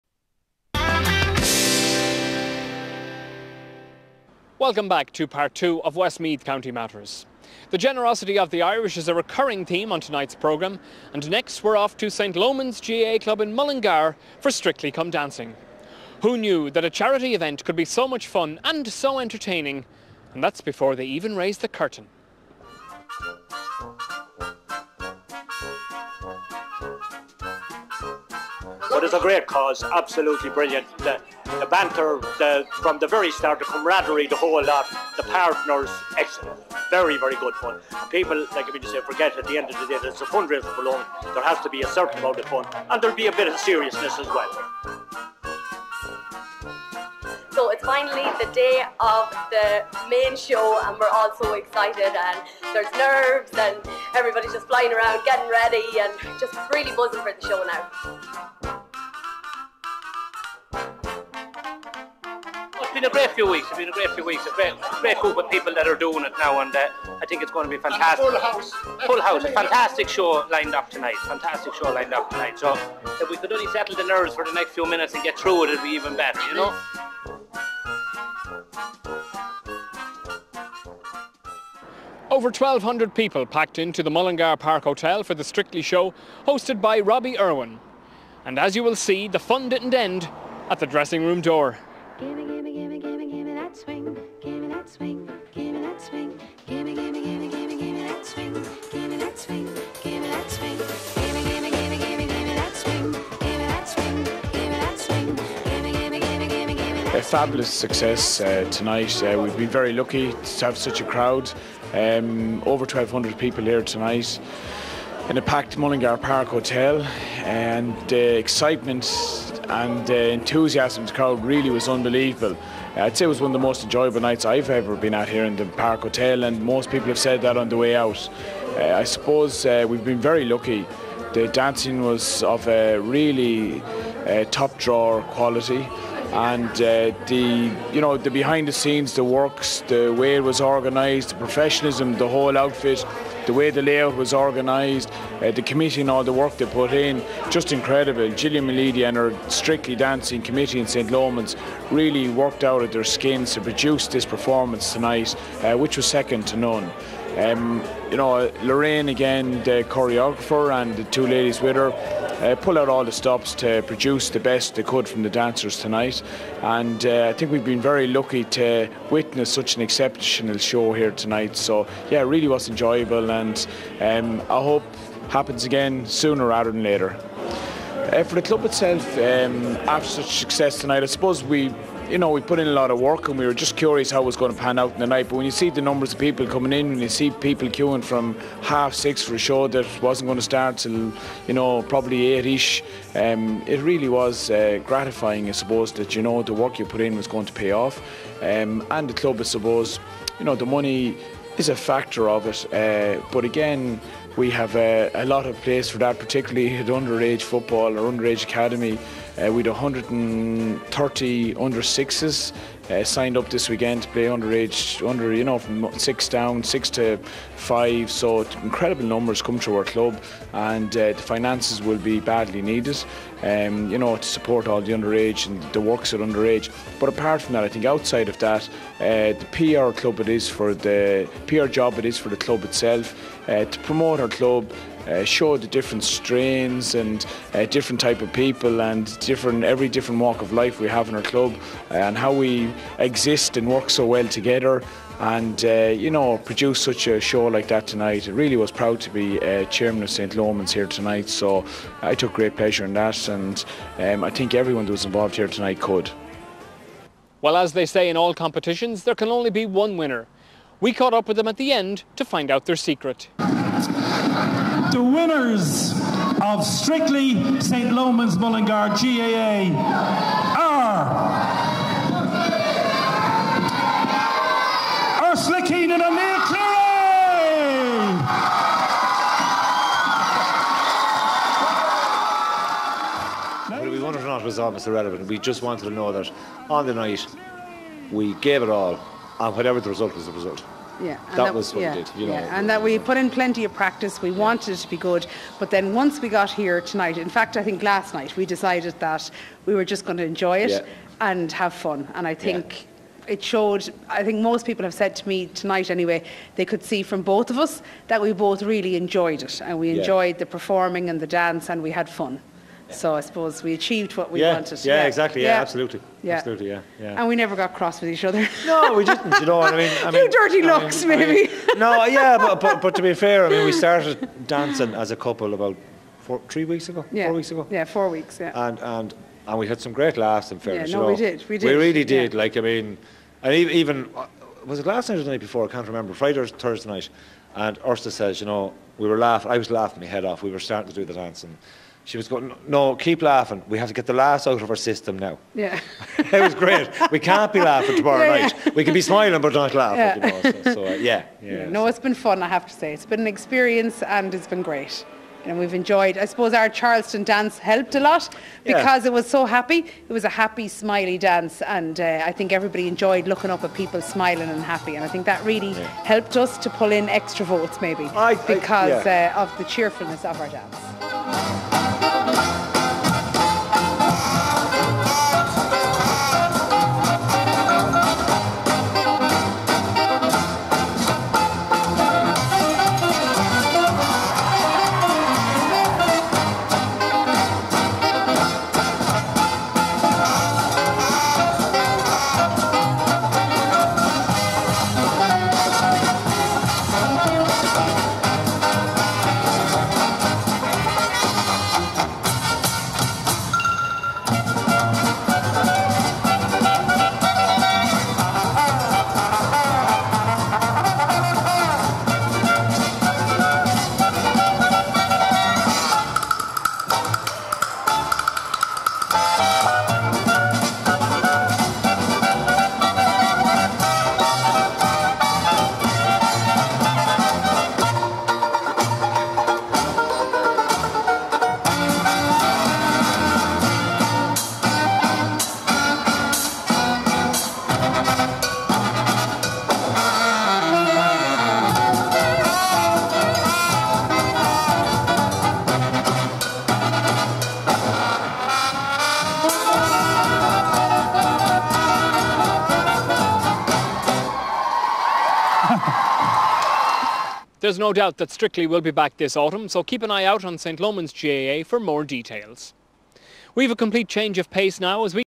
St Lomans strictly come dancing.
westmeath county matters, charity event at St Lomans GAA Strictly come dancing. 1200 people turned up to the event at the Park hotel in Mullingar. Winners Ursula Keenan and Neil Clearly steal the show in a most memorable night